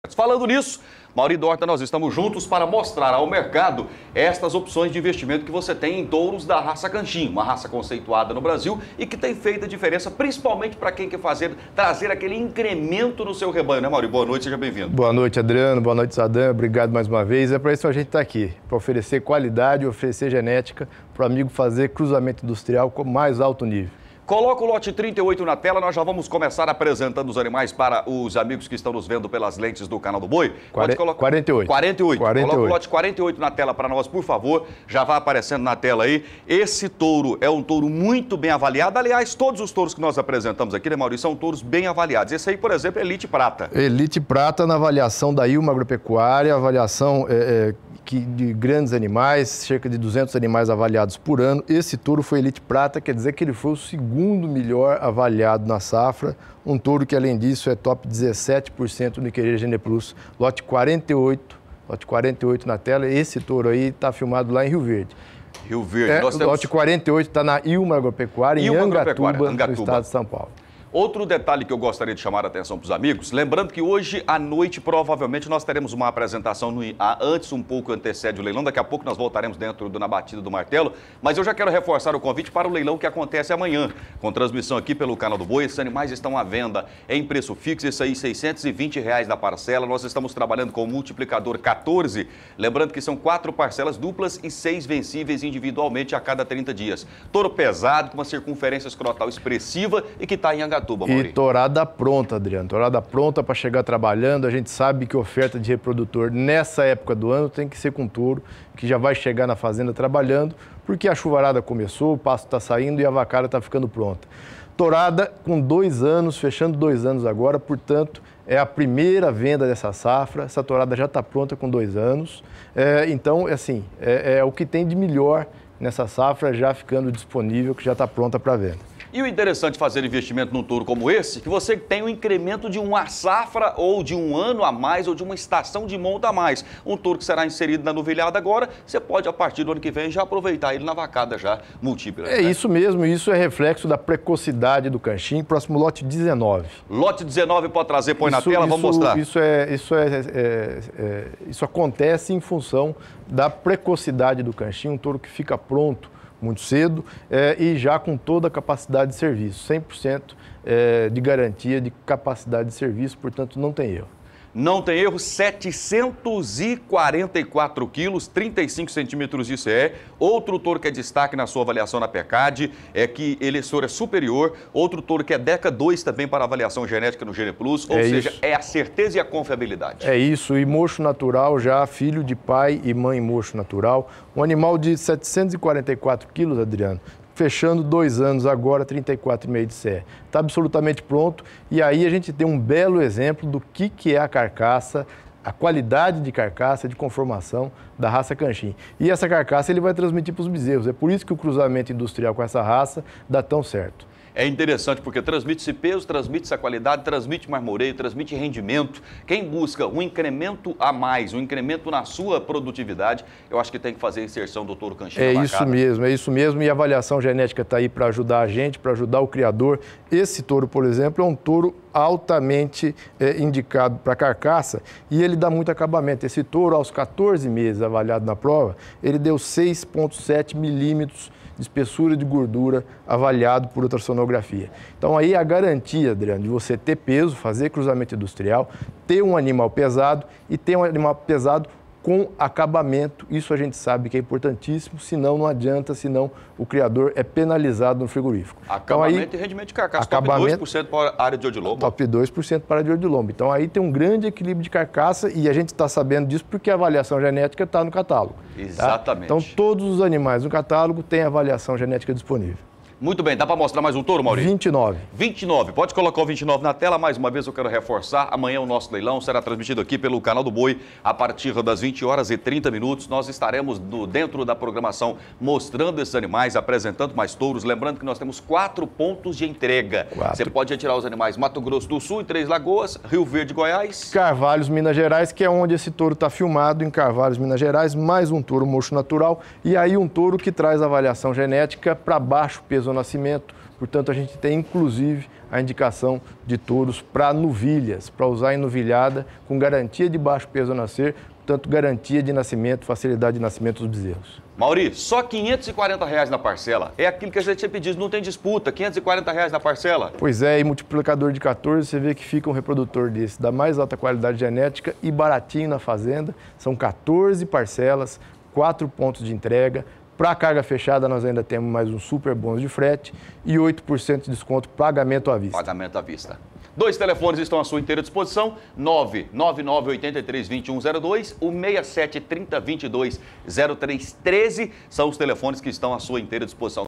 DE OLHO NA FAZENDA ESSÊNCIA DA PRODUÇÃO TOUROS CANCHIM
O quadro De Olho na Fazenda - Essência da Produção- que coloca a disposição do mercado reprodutores da raça Canchim. Uma realização de 4 grandes criatórios: Ilma Agropecuária, Canchim Itamarati , Fazenda São Tomé e Fazenda São Joaquim.